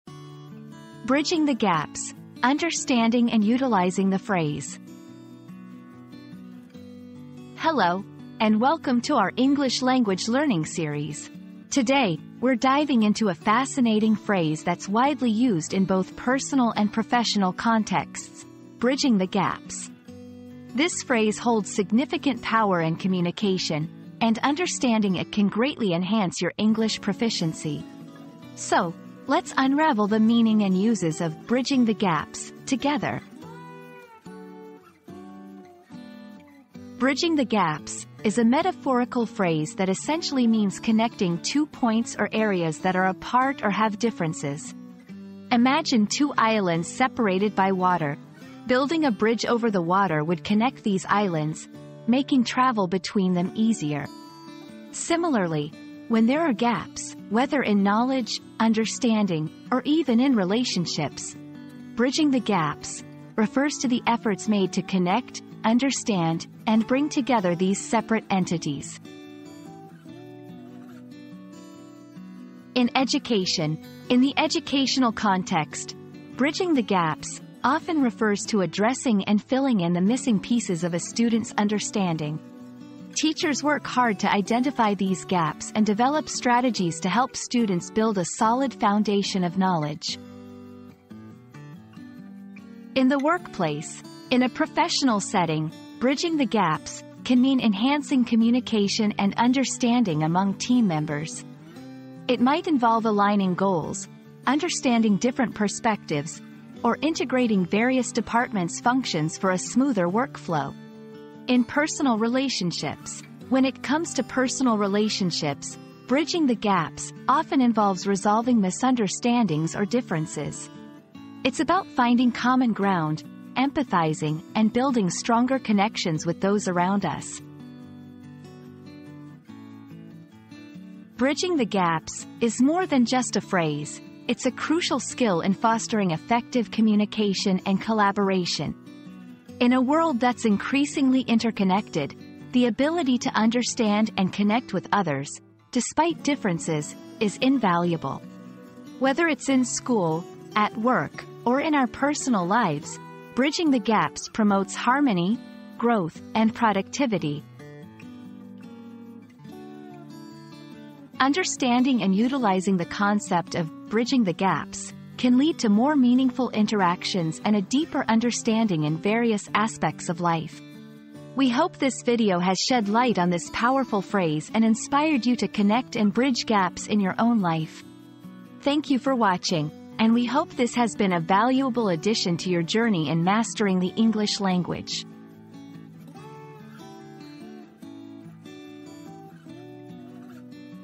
Bridging the Gaps: Understanding and Utilizing the Phrase
00:00 • Introduction - Bridging the Gaps: Understanding and Utilizing the Phrase
00:36 • What Does "Bridging the Gaps" Mean?
01:12 • Usage of "Bridging the Gaps" in Different Contexts
02:10 • Why is "Bridging the Gaps" Important?